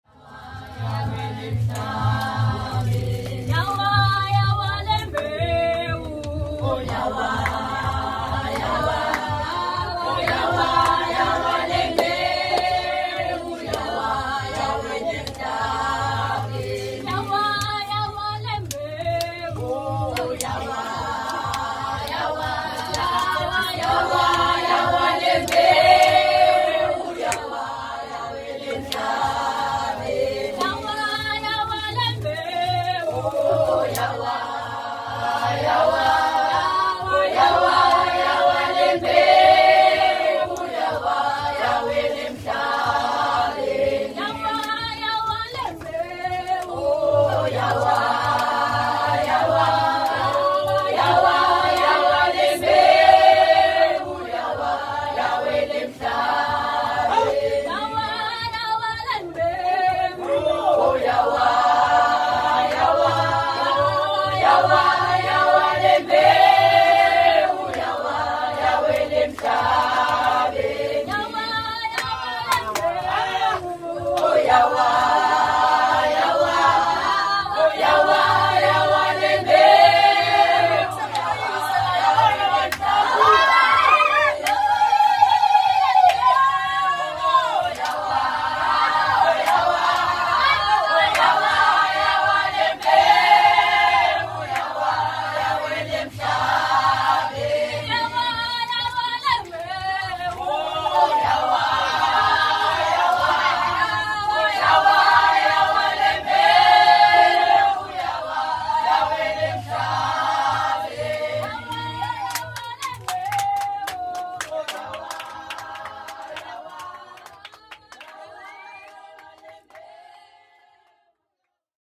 Special song of Youth, African Township Church #2 / 아틀란티스 교회 청소년들의 아프리카 찬양 특송 #2 /
케이프타운에서 중심지에서 서북쪽 해안선을 따로 약 80km 떨어진 거리에
아틀란티스라는 지명이 있다. 그리고 지도에 나오지 않는이름,
미개발지역 마을을 개척해 섬기고 있다.
원래는 마을 중심에 있는 술을 파는 pup&bar 이었는데,
그곳을 구입해서 교회를 개척했다.
현재 아름답게 성장하고 있고 매 주일마다 하나님의 열심으로
은혜로운 예배를 드리고 있다.